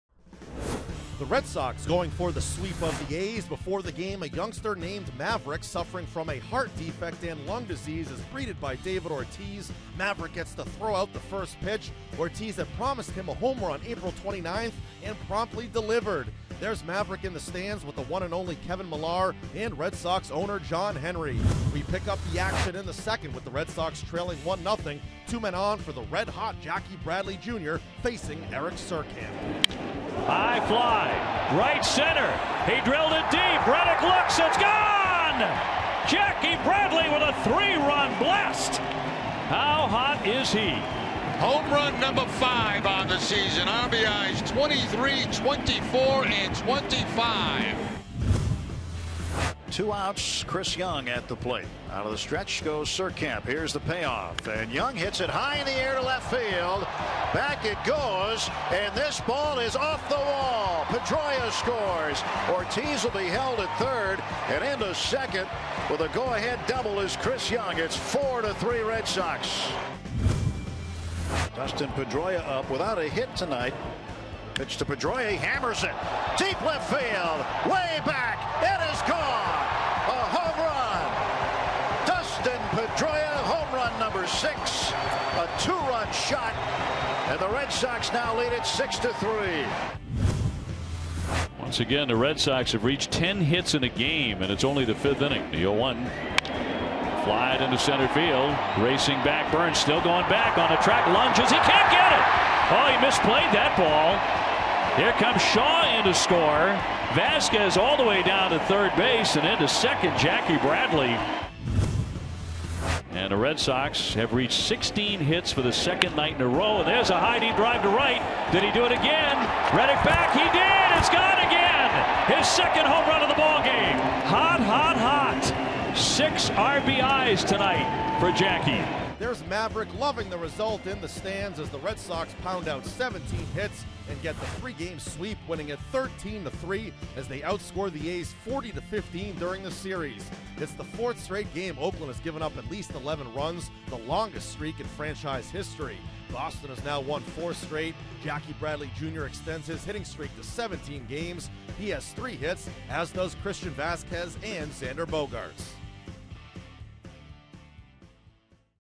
5/11/16: Red Sox power their way past Athletics
Daily Recap: Jackie Bradley Jr. had six RBIs and two home runs as the Red Sox collected 17 hits in a 13-3 victory over the Athletics